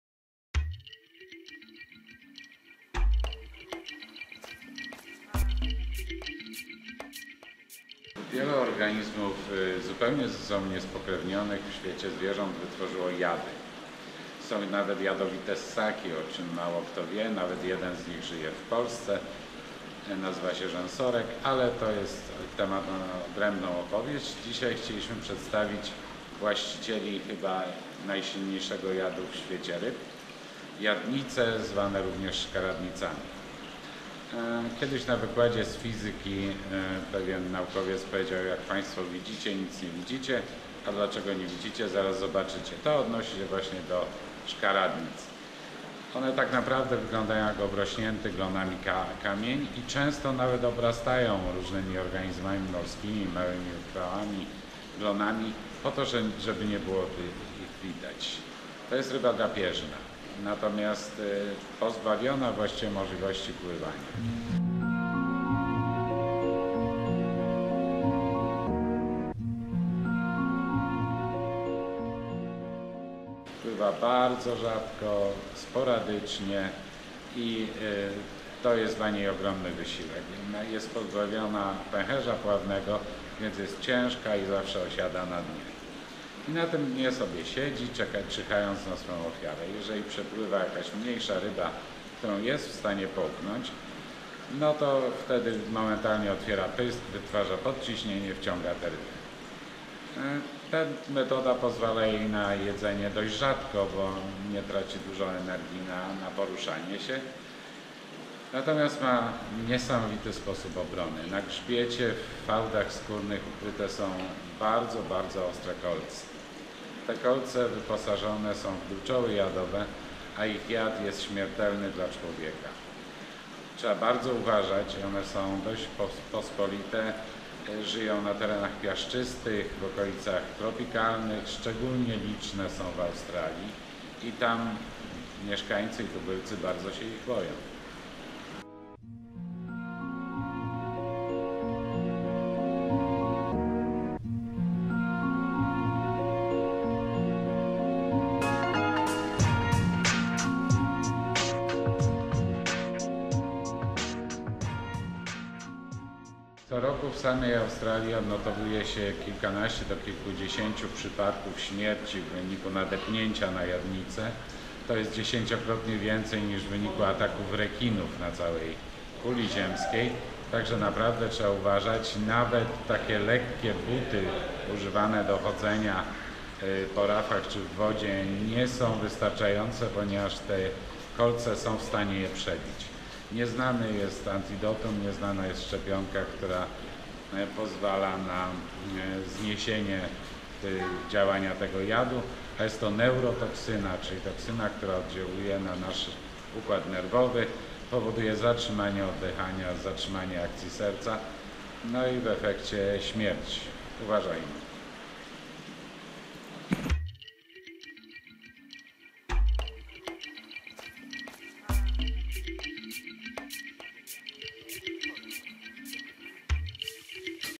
Odcinek 33 "Jadowity kamień"
Jadowita ryba udająca kamień?  W dzisiejszym odcinku Szlakiem Ratajszczaka, posłuchajcie opowieści o szkaradnicy.